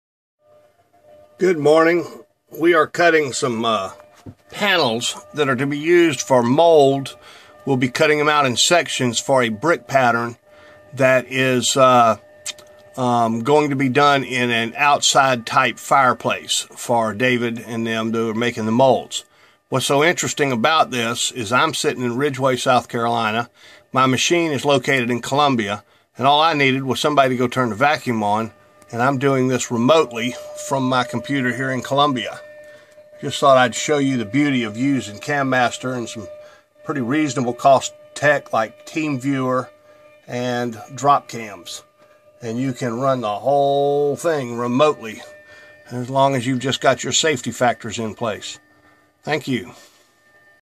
Camaster Remote controll
Using Team viewer and Dropcams to run panels on a sunday while i am home at the Farm